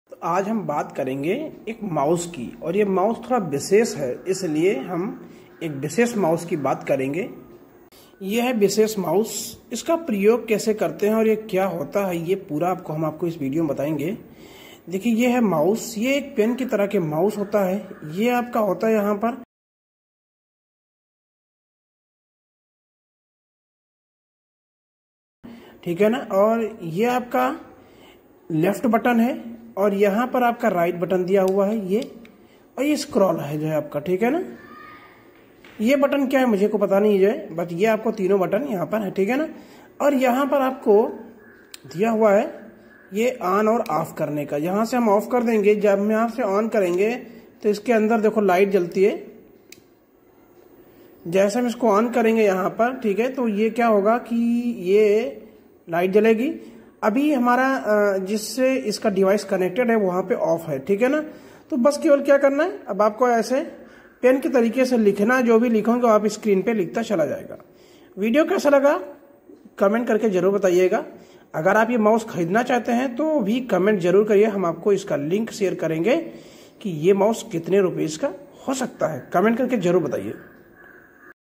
Pen Mouse | What is Pen Mouse | RITI | RITI TECHNOLOGY
Is video mein ham pen mouse ke bare mein baat karenge yah normal Mouse se alag Hota Hai yah pen Jaisa Dikhai padta hai jaane ke liye pura video Dekhen aur comment Karke bataiye video kaisa laga dhanyvad